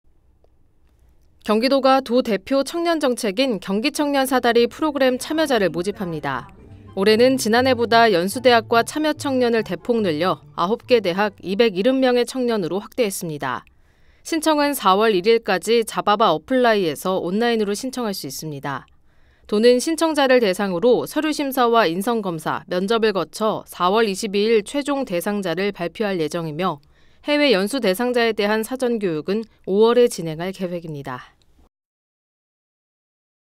‘경기청년 사다리’ 참여자 모집
경기도가 도 대표 청년 정책인 ‘경기청년 사다리’ 프로그램 참여자를 모집합니다.

올해는 지난해보다 연수 대학과 참여 청년을 대폭 늘려 9개 대학, 270명의 청년으로 확대했습니다.

신청은 4월 1일까지 잡아바 어플라이에서 온라인으로 신청할 수 있습니다.

도는 신청자를 대상으로 서류 심사와 인성 검사, 면접을 거쳐 4월 22일 최종 대상자를 발표할 예정이며 해외 연수 대상자에 대한 사전 교육은 5월에 진행할 계획입니다.